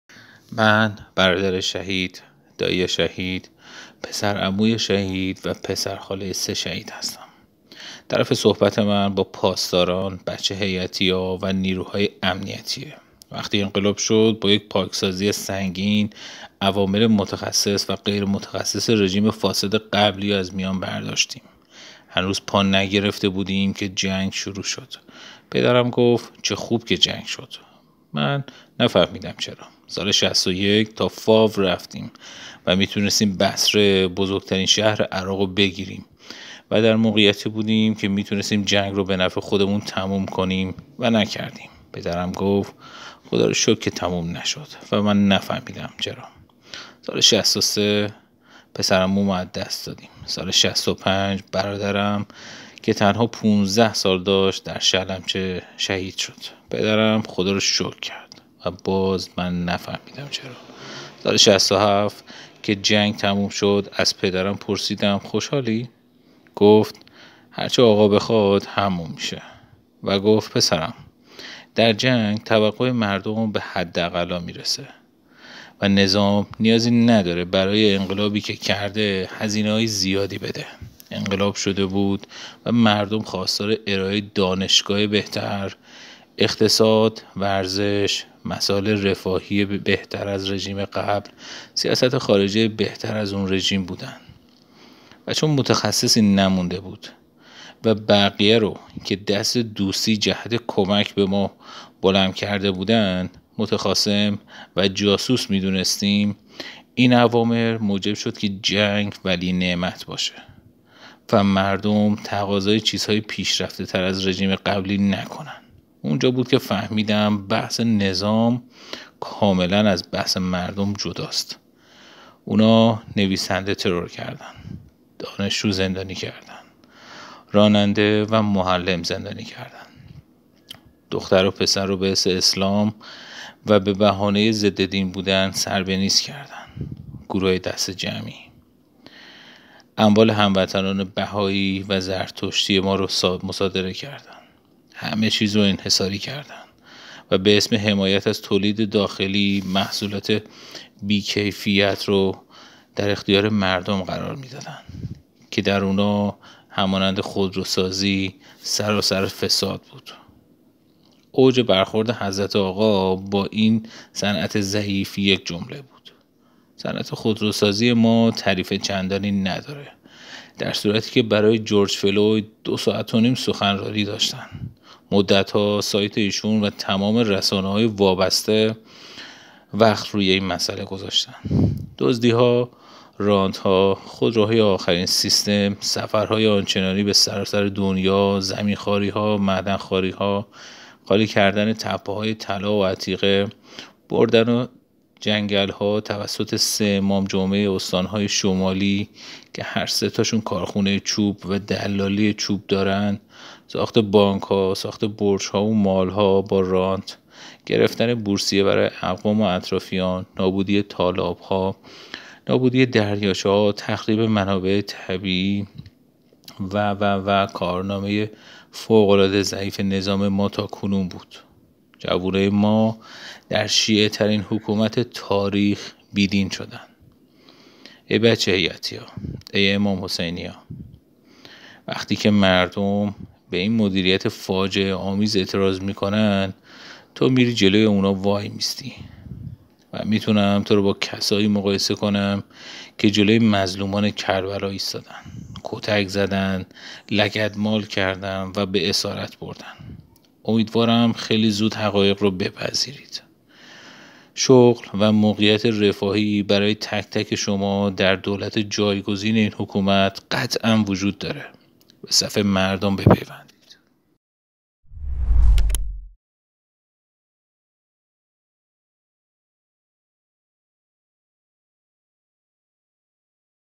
پیام برادر شهید به بسیجی: به صف مردم بپیوندید
«من برادر شهید، دایی شهید، پسر عمو و پسر خاله شهیدان هستم.
بابام بعد شهید شدن برادرم حسابی طرفدار حکومت شد.
ولی من و برادرام راهمونو از این نظام و پدرم جدا کردیم.
ما به ماهیت این نظام پی بردیم..
می‌‌خواستم پیام تصویری بذارم ولی بخاطر پدر که سن بالایی دارن نذاشتم.
و می‌خوام در یک پیام صوتی خطاب به بچه بسیجیا مطلبی رو بگم»